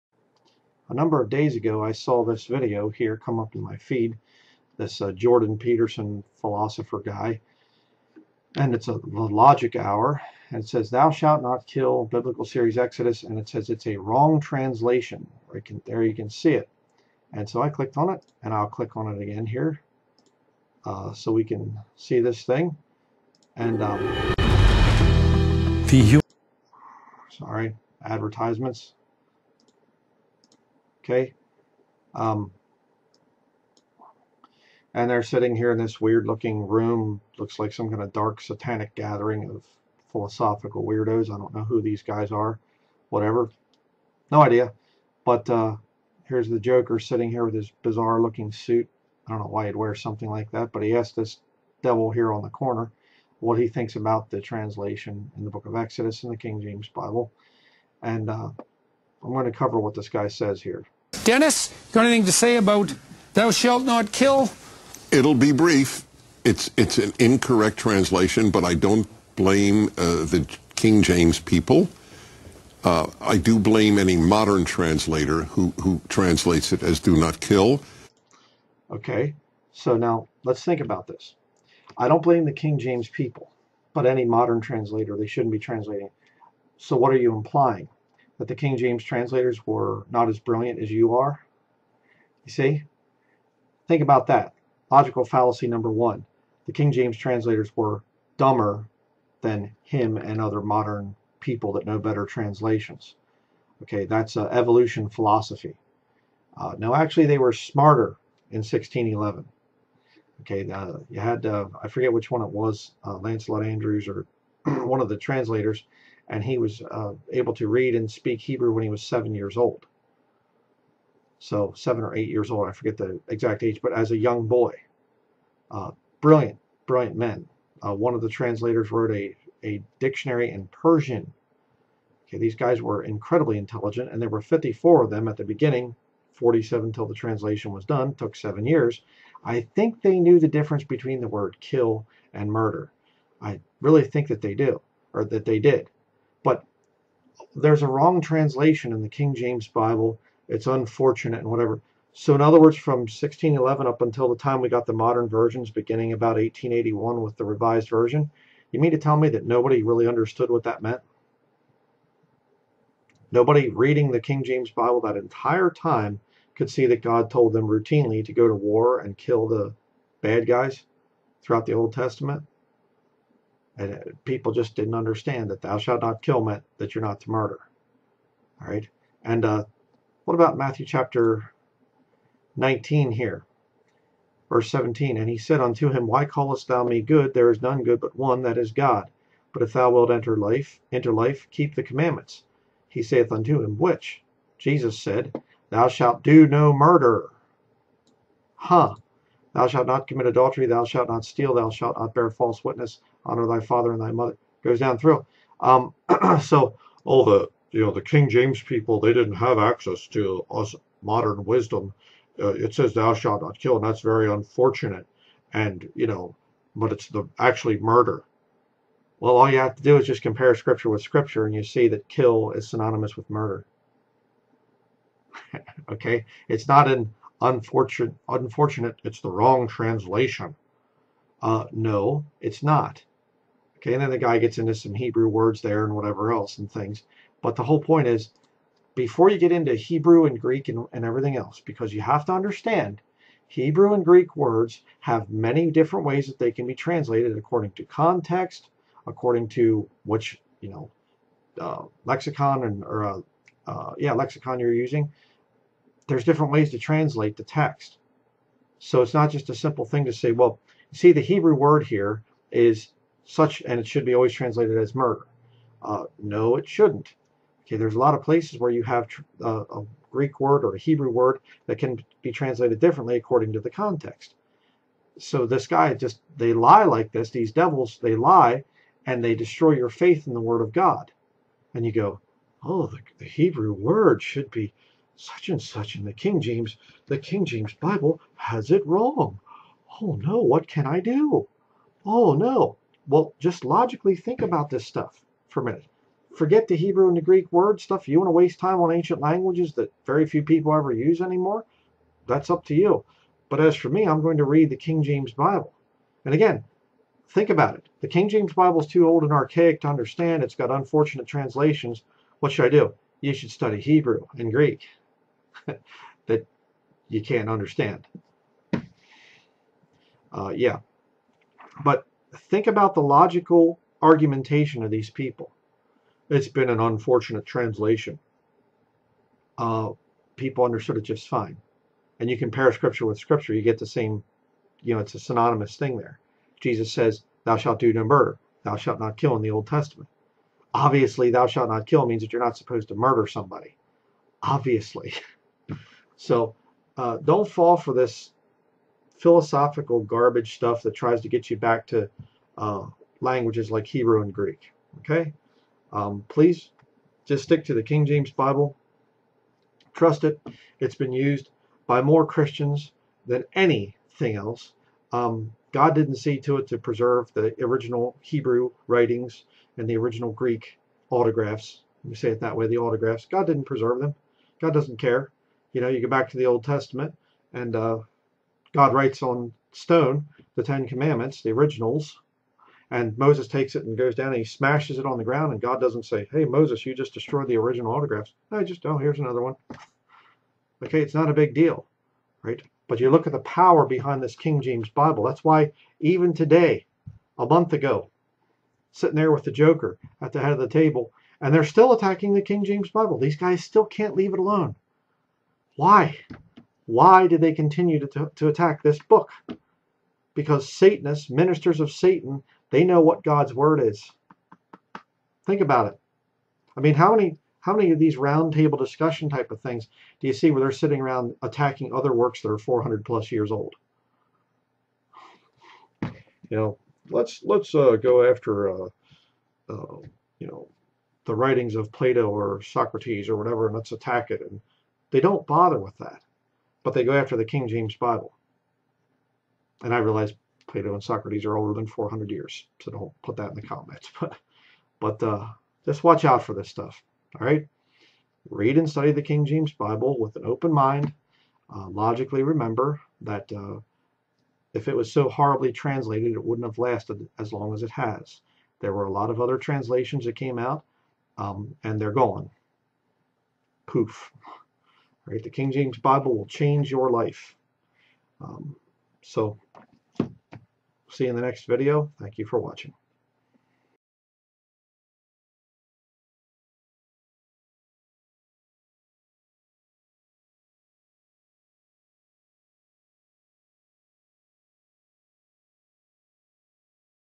Jordan Peterson's Philosopher Buddy Rejecting The KJV
Yet another servant of the devil, trying to destroy people's faith in the King James Bible. As usual, he has no real arguments against God's Holy written word.